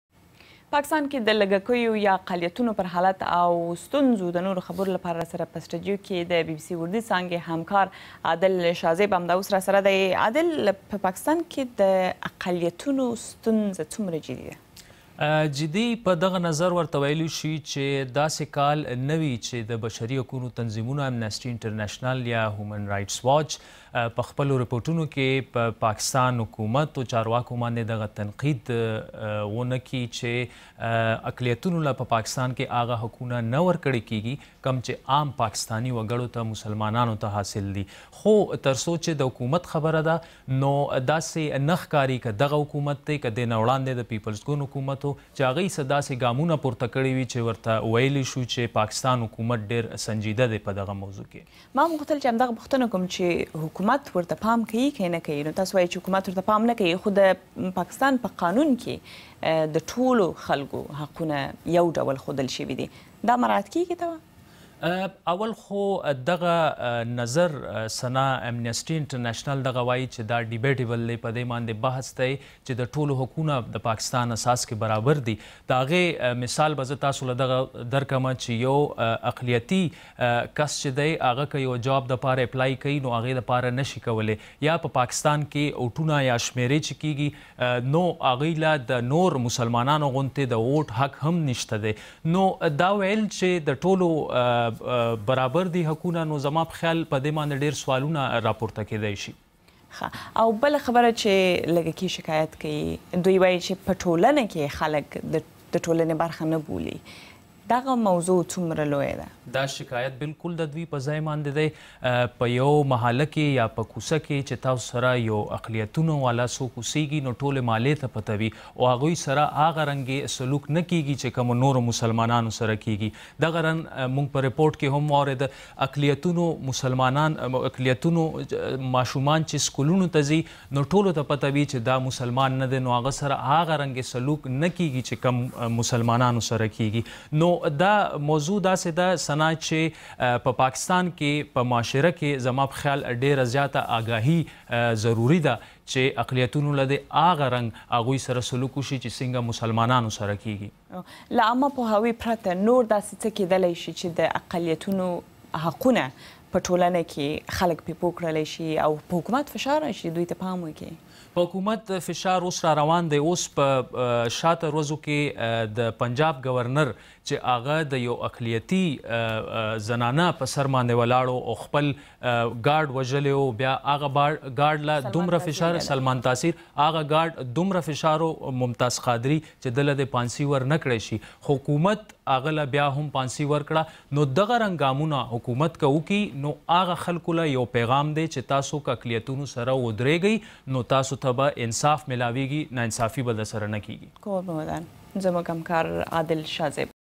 The Plight of religious minorities in Pakistan
The Plight of religious minorities in Pakistan. BBC Pashto's Sana Safi speaks to Adil Shahzeb